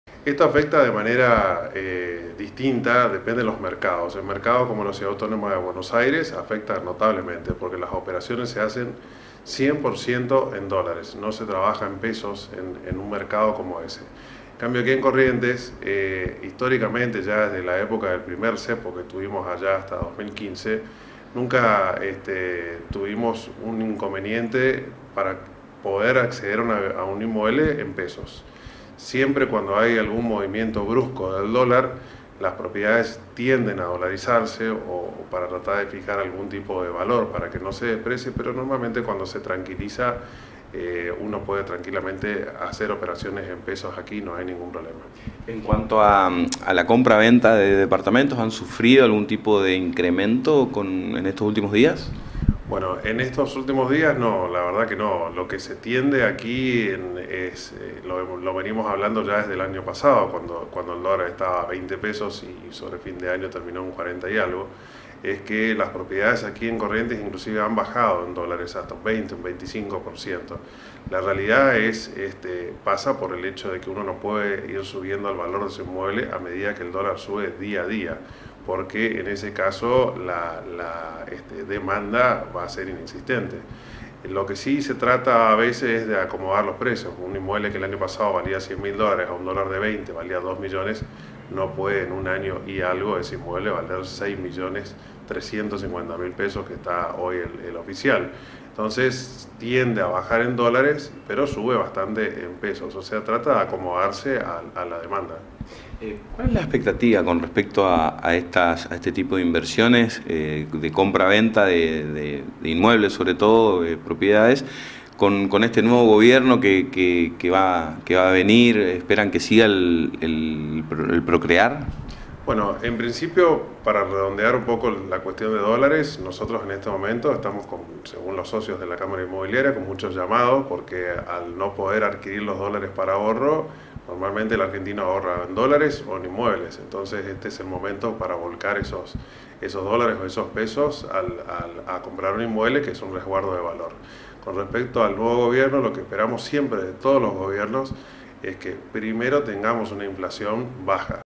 C9 - CORRIENTES - INCERTIDUMBRE INMOBILIARIA
MEJORARON LAS TRANSACCIONES ANTE EL CEPO CAMBIARIO
 En el marco del próximo cambio de Gobierno nacional y la crisis económica, el presidente de la Cámara Inmobiliaria de Corrientes, Iván Montanaro, dijo que aumentó la última semana el número de transacciones inmobiliarias ya que “es una forma de proteger el dinero”. Esto se da por endurecimiento del cepo al dólar, no se puede comprar en gran cantidad la moneda estadounidense y, la inestabilidad económica que lleva a que, el que tenga dinero en el banco, lo retire y “compre ladrillo”.
IVAN MONTANARO-PRES.CÁMARA INMOB.CTS.